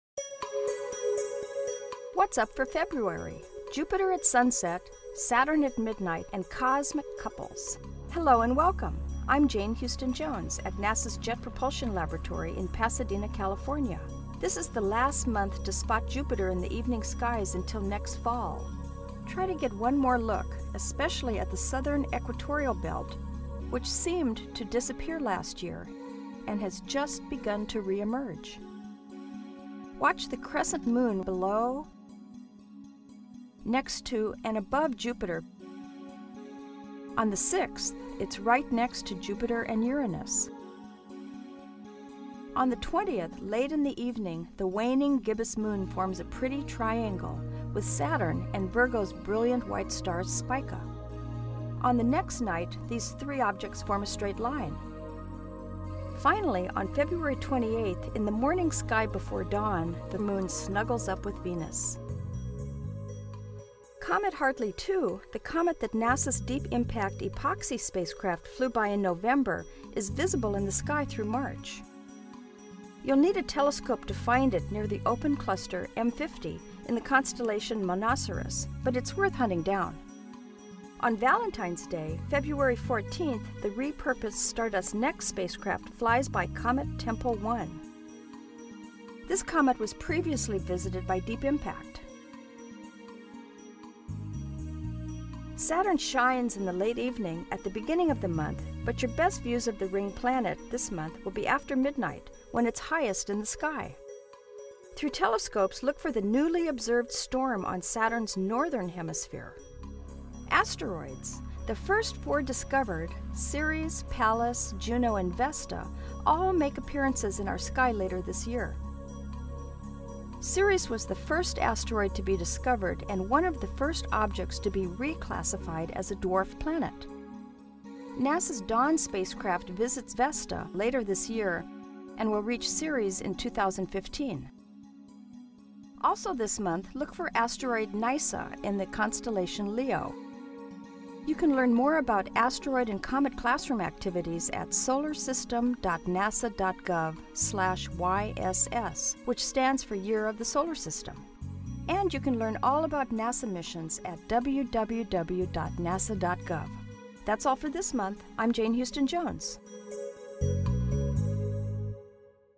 NASA: What's Up For February 2011? [720p]
This month see Jupiter at sunset, Saturn at midnight, pretty moon and planet couples in-between.

credit: NASA/JPL/Caltech